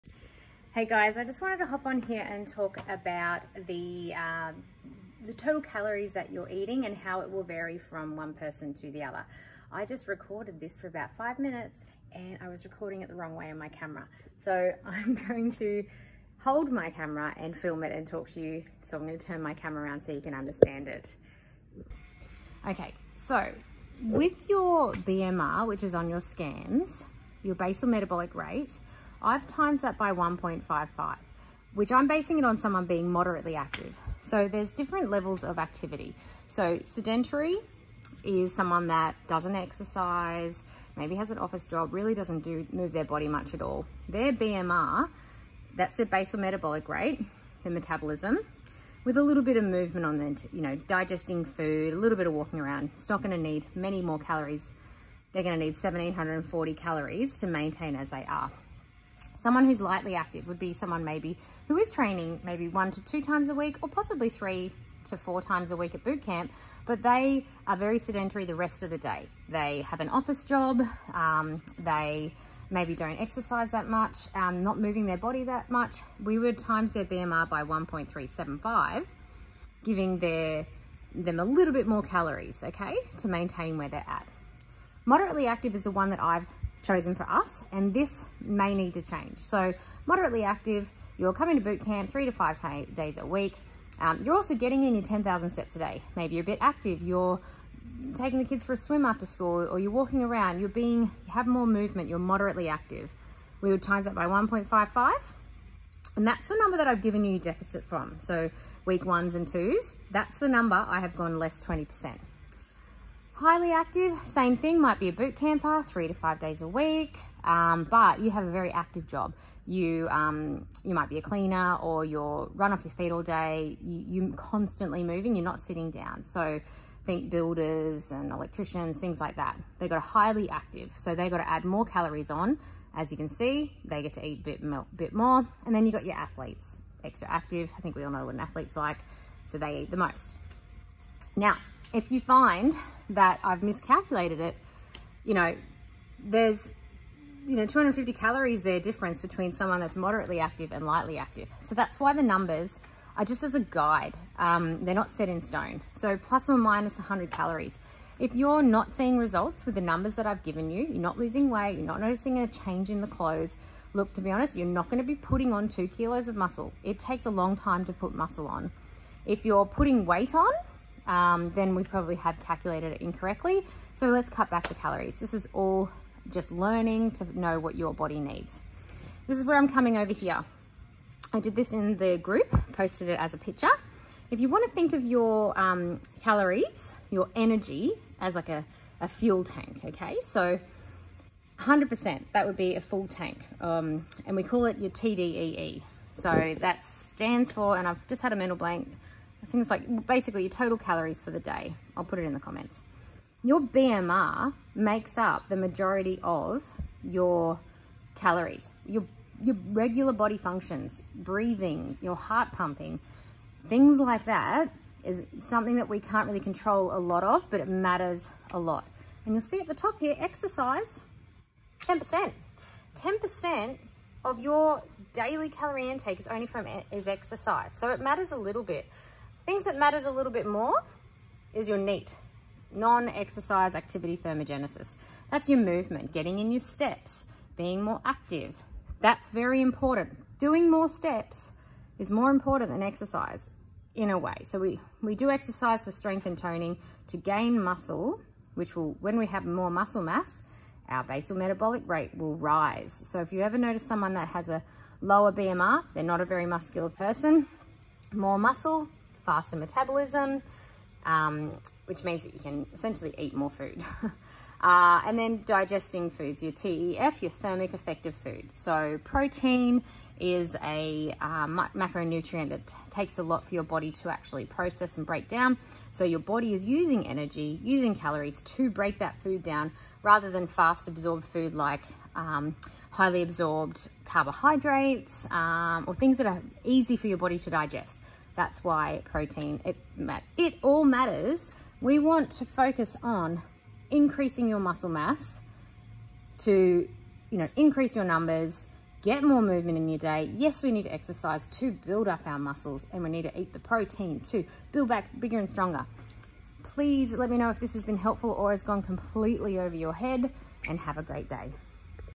What your TDEE breakdown and how to know if you’re eating calories right
Your Total Daily Energy Expenditure (TDEE) is an estimation of how many calories you burn per day when exercise is taken into account

Let’s see if we are calculating right